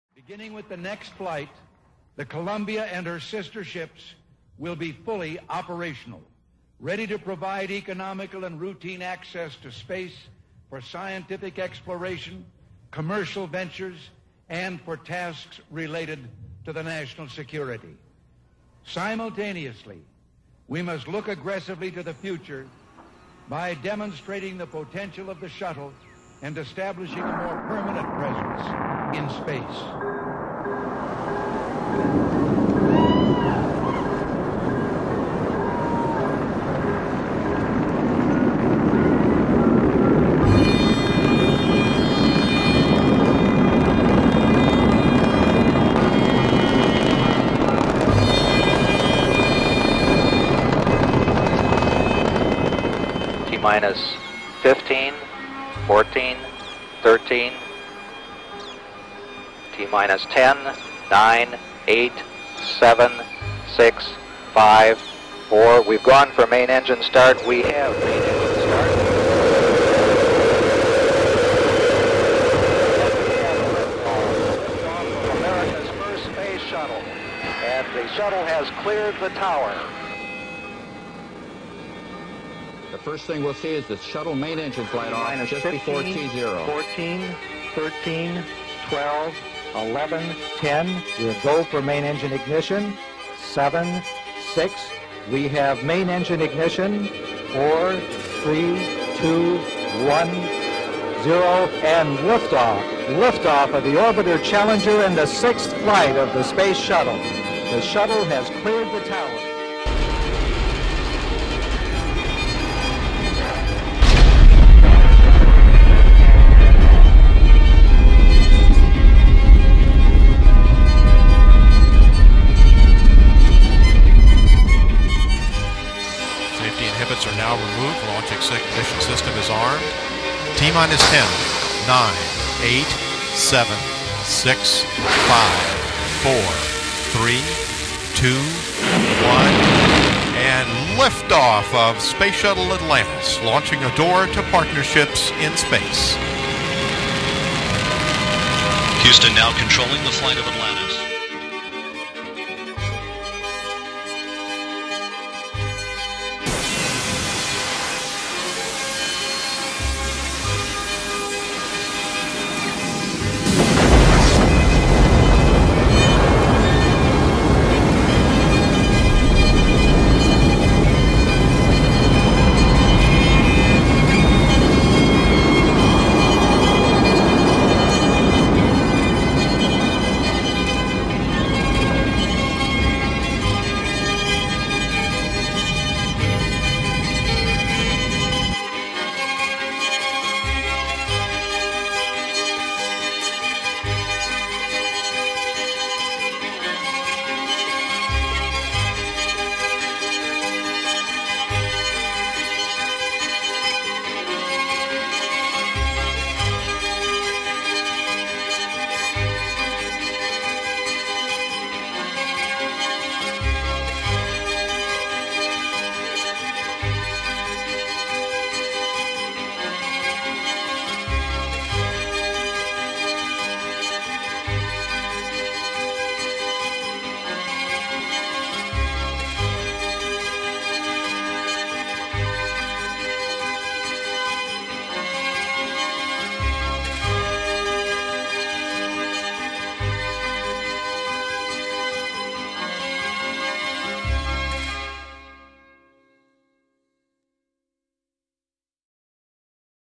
The Space Shuttle Fleet
Avaliable in HD. After 30 years of service, The Space Shuttle remains the most advanced piece of engineering hardware humanity has ever created.

Columbia, Challenger, Discovery, Atlantis, Endeavour.

Using clips from some of the best archival footage available, see the power of what science and technology can accomplish.

Source: NASA/IMAX